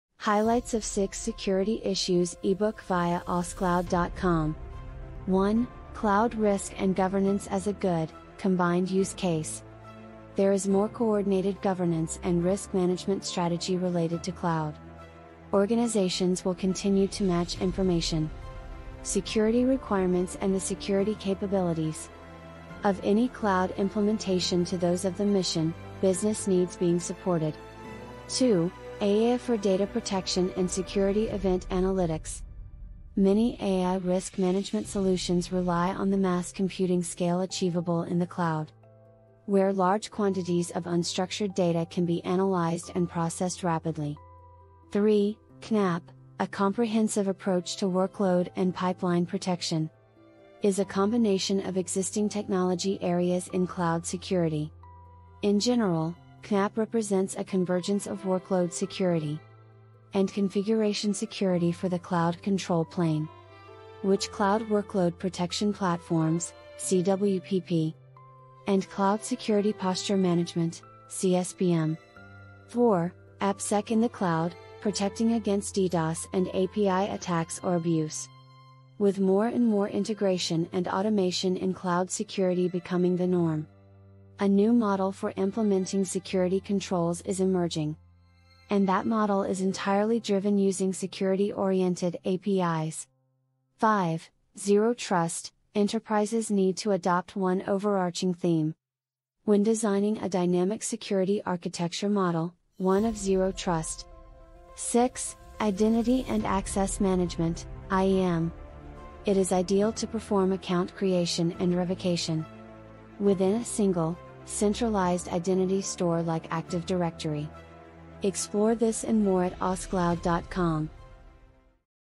Highlights of 6 Security Issues eBook via Awscloud.com
1 - Cloud Risk and Governance as a Good, Combined Use Case
There is more coordinated governance and risk management strategy related to cloud. Organizations will continue to match information,
 security requirements and the security capabilities,
 of any cloud implementation to those of the mission/business needs being supported.
2 - AI for Data Protection and Security Event Analytics.
Many AI risk management solutions rely on the mass computing scale
achievable in the cloud,
where large quantities of unstructured data can be analyzed and processed rapidly.
3 - CNAPP: A Comprehensive Approach to Workload and Pipeline Protection,
is a combination of existing technology areas in cloud security.
In general, CNAPP represents a convergence of workload security,
 and configuration security for the cloud control plane,
which cloud workload protection platforms (CWPP),
 and cloud security posture management (CSPM) already cover.
4 - AppSec in the Cloud: Protecting Against DDoS and API Attacks or Abuse
 With more and more integration and automation in cloud security becoming the norm,
 a new model for implementing security controls is emerging,
and that model is entirely driven using security-oriented APIs.
5 - Zero Trust - enterprises need to adopt one overarching theme,
 when designing a dynamic security architecture model—one of “zero trust”.
6 - Identity and Access Management (IAM).
It is ideal to perform account creation and revocation,
 within a single, centralized identity store like Active Directory.
Explore this and more at Awscloud.com